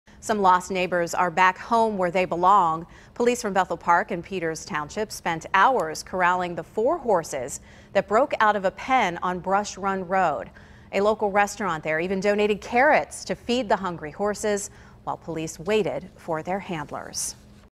Bethel Park, Peters Township Police Team Up On Horse-Hunt
The Bethel Park and Peters Township police departments took horsing around to a new level on Saturday.